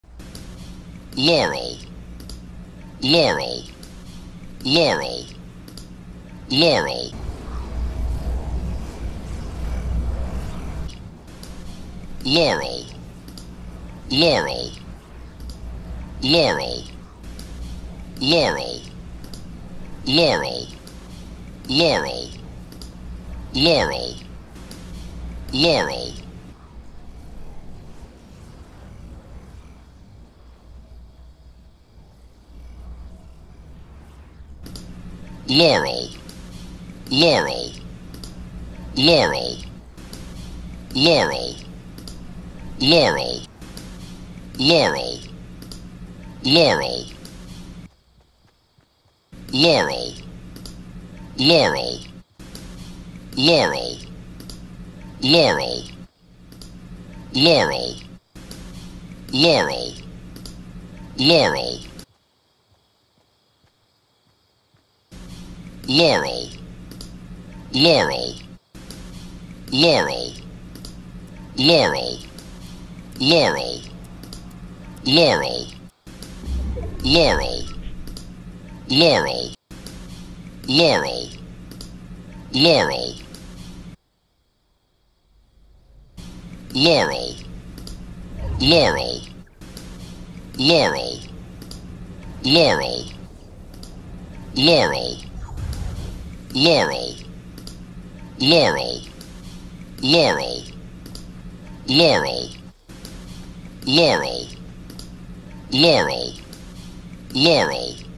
Do You Hear “Yanny” or “Laurel” Challenge
Yanny or Laurel ear trick is taking the internet by storm. DO IT WITH FRIENDS and you'll see some of you hear something entirely different!  Evidently, if you can hear high frequencies, you hear Laurel, otherwise you hear Yanni. WILD!!

Game making, animation, pixel art, music and tutorials.